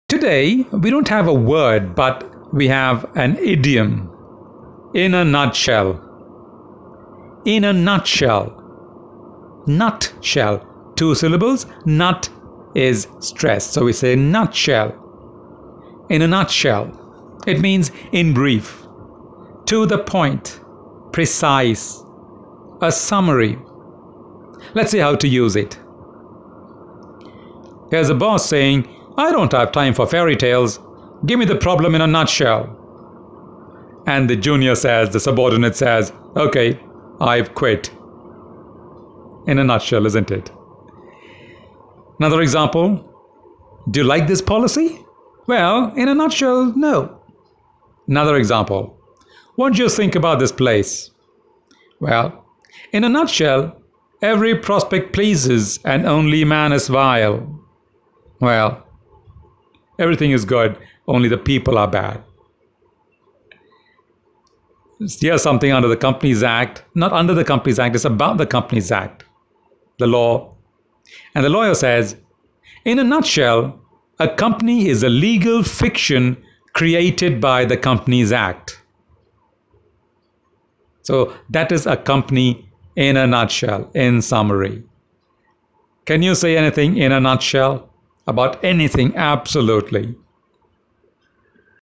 In a nutshell | Meaning of In a nutshell | "In A Nutshell" in a sentence | nutshell synonyms
Learn how to pronounce and use English words correctly.
When you wish to say something in brief, or in a summary, you say IN A NUTSHELL.

You can easily draw a picture here: a nutshell is a small place where you can keep only a very small thing. So, when you say in a nutshell, you say it in brief.

A “pictorial idiom” where the listener can easily draw the picture of what you are saying. English has a lot of such “pictorial” idioms that you can use to clarify your expressions and make them precise and comprehensible.